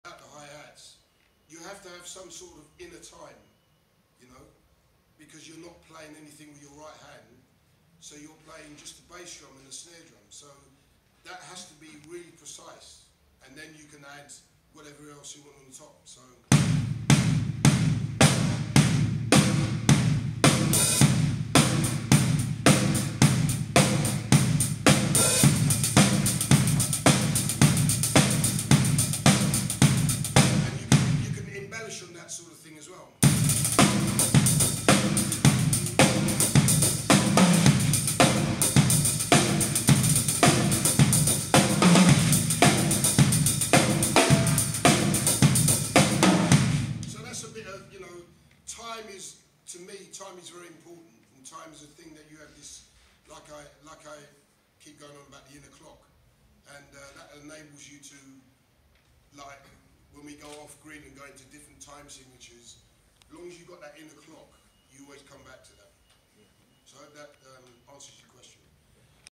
Mel Gaynor during his personal clinic at the UK Drum Show in Liverpool on October 1st.
Mel talked about his rhythms, the importance of time in his most famous grooves with Simple Minds.